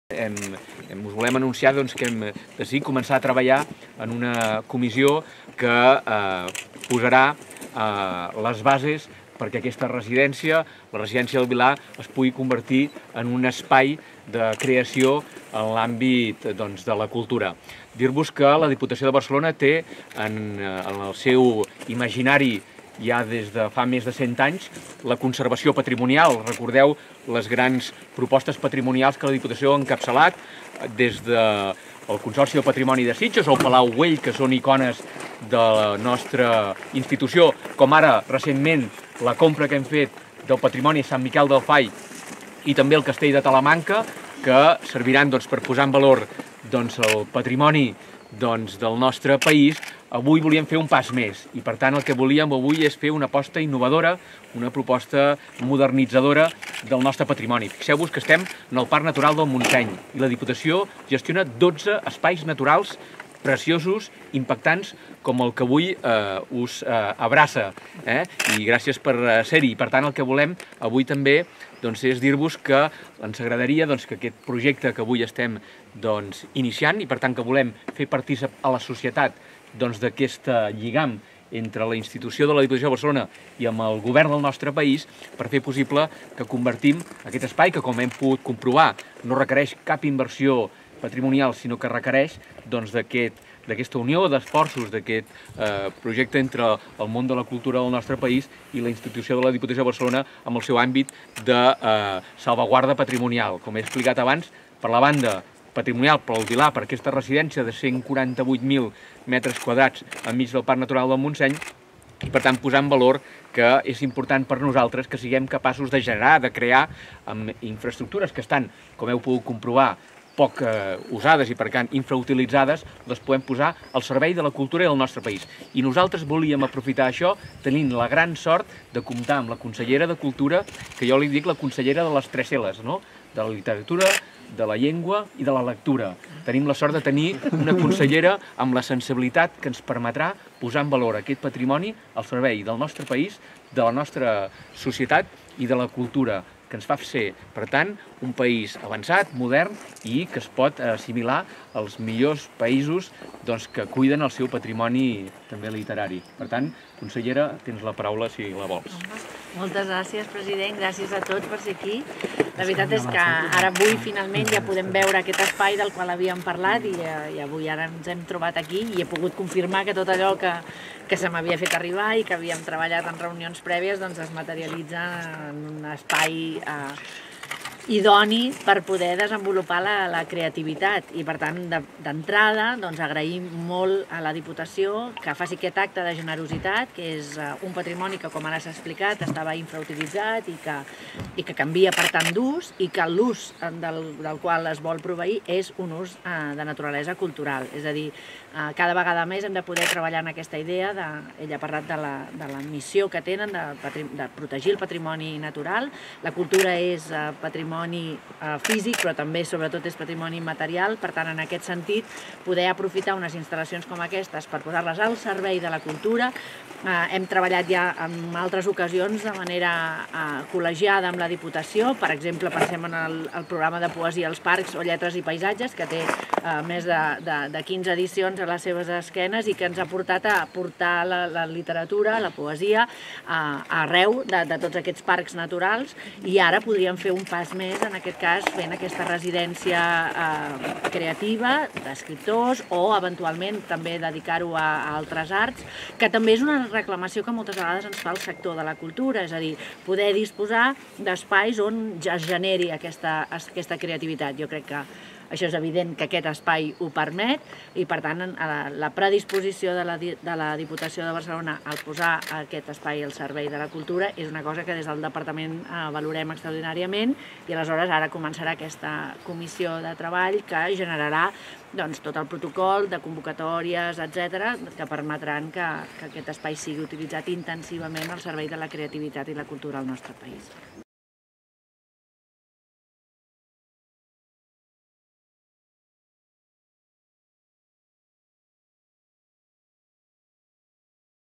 La Diputació habilitarà al Montseny un nou espai de creació per a escriptors i artistes
El president de la Diputació de Barcelona, Marc Castells, i la consellera de Cultura de la Generalitat de Catalunya, Laura Borràs, han visitat el 8 de febrer de 2019 la finca del Vilar, a Fogars de Montclús. Durant la visita han explicat els futurs usos de l’equipament, propietat de la Diputació de Barcelona, dins el Parc Natural – Reserva de la Biosfera del Montseny.

La finca acollirà un nou espai de creació per a escriptors i artistes, un nou equipament a disposició de la societat i de la cultura del país, en una aposta innovadora i modernitzadora del nostre patrimoni, i amb l’acompanyament de la Generalitat, ha explicat el president de la Diputació.